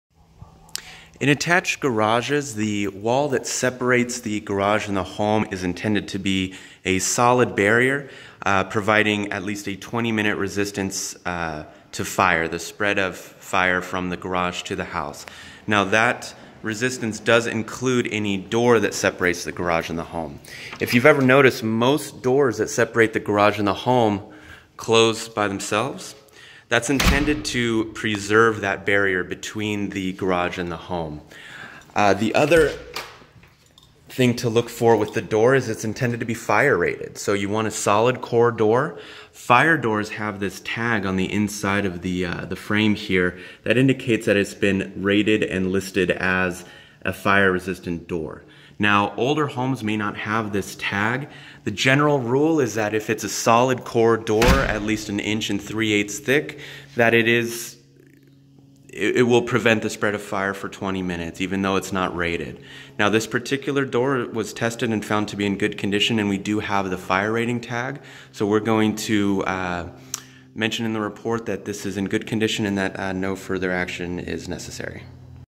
Inspect-A-Minute! - Garage Fire Door
The door between an attached garage and the home should be rated for 20 minute fire resistance and should self close.